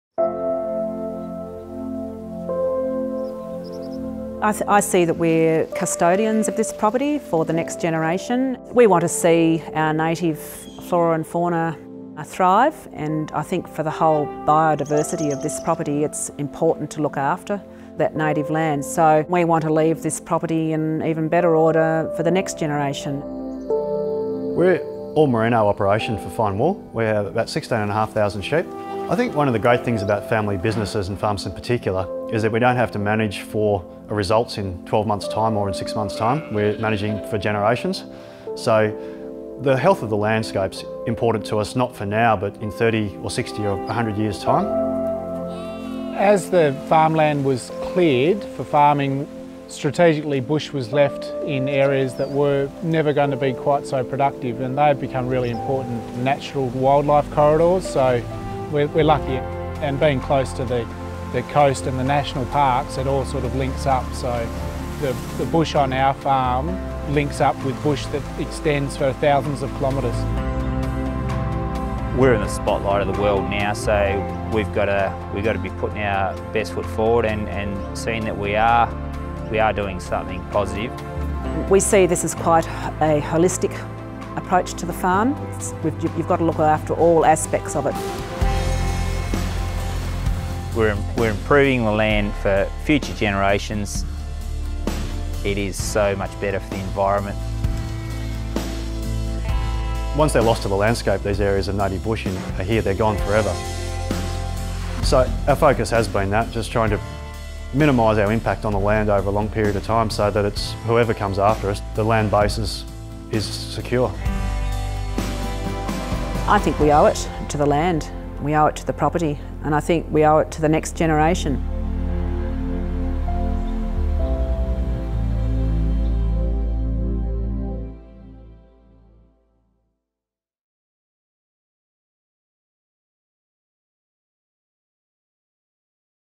The Woolmark Company presents WOOLGROWER SPOTLIGHT: An Environmental Series
Australian Merino farmers work tirelessly to look after their sheep and the land on which they graze, as the wellbeing of their animals is of utmost importance, and this also, quite simply, produces the best wool. So, it's in the interests of the Merino farmers to commit to exceptionally high standards of animal welfare, responsible use of land and water resources, and sustainable farming practices for the benefit of their animals, their families, communities and of the earth.
Join us as we tour some of Australia's wool-growing properties in this Grower Spotlight series and meet the true custodians of the land.